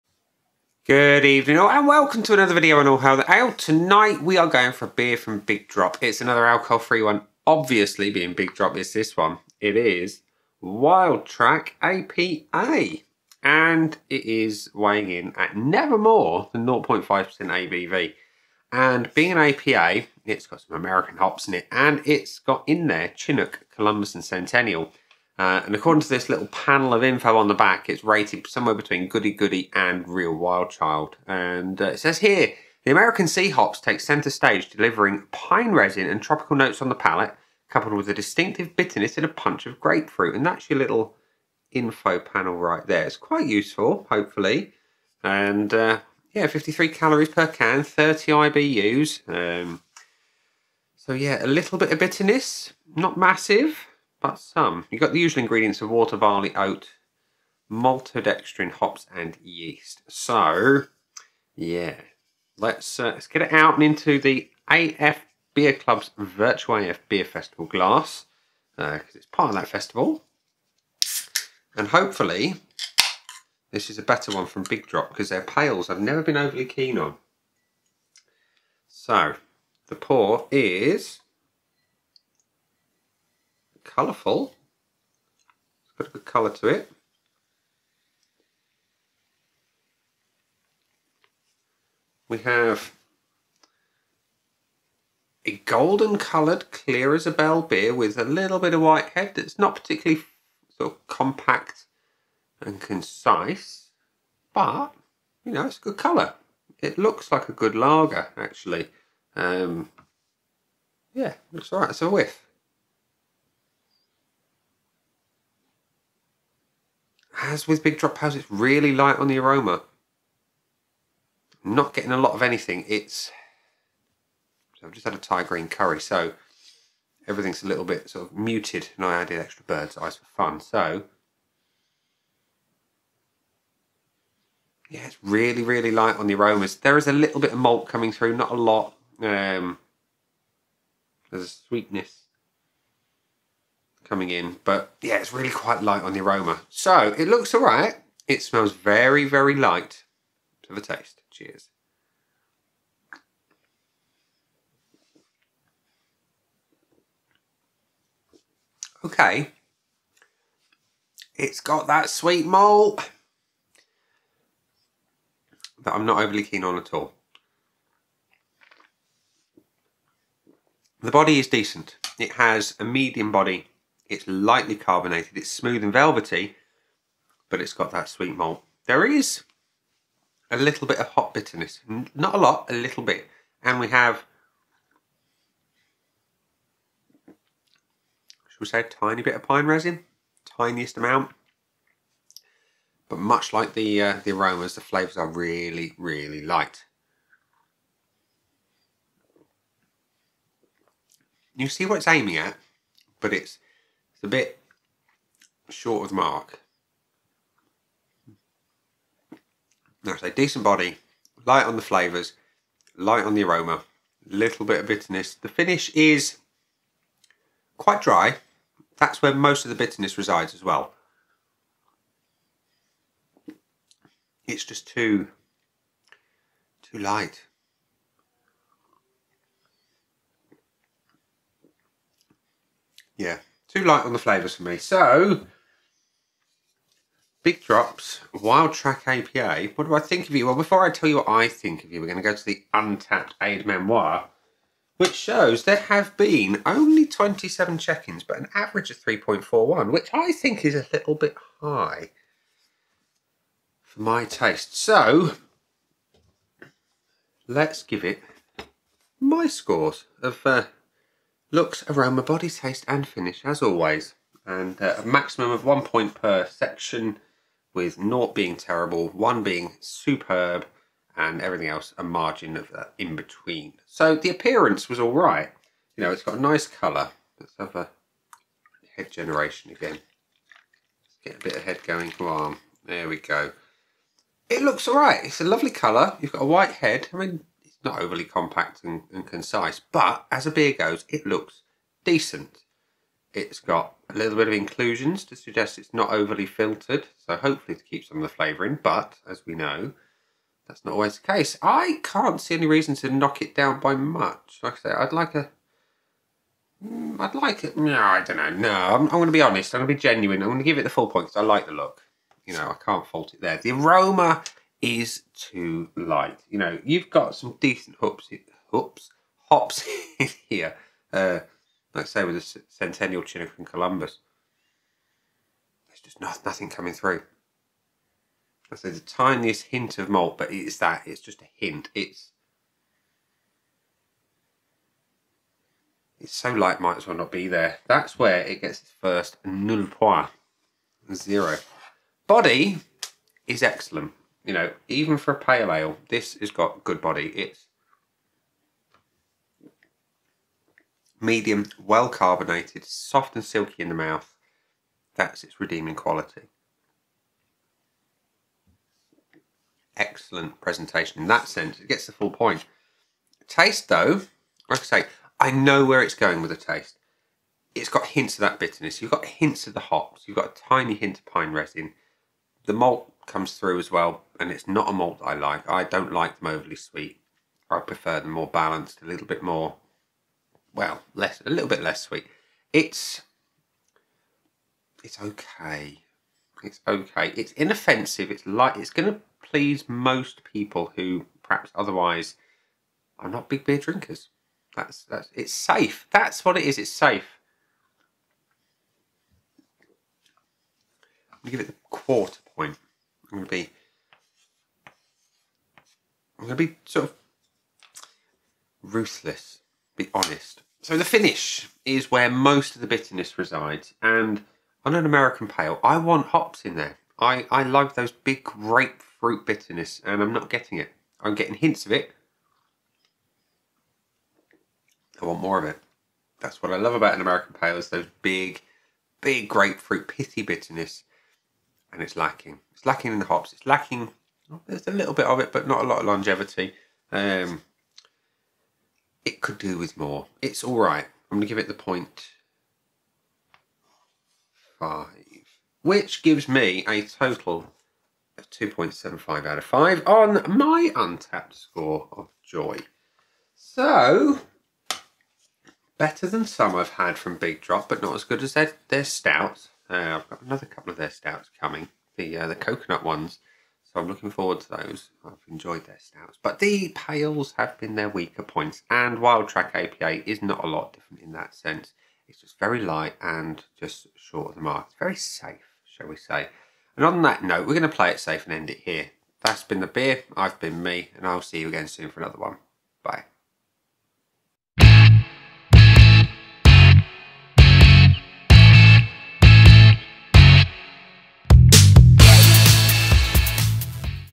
BIG DROP - Wildtrack APA Review
Another beer from Big Drop, this time it is their Wildtrack APA.

Brewery: Big Drop
Beer: Wildtrack APA
ABV: 0.5%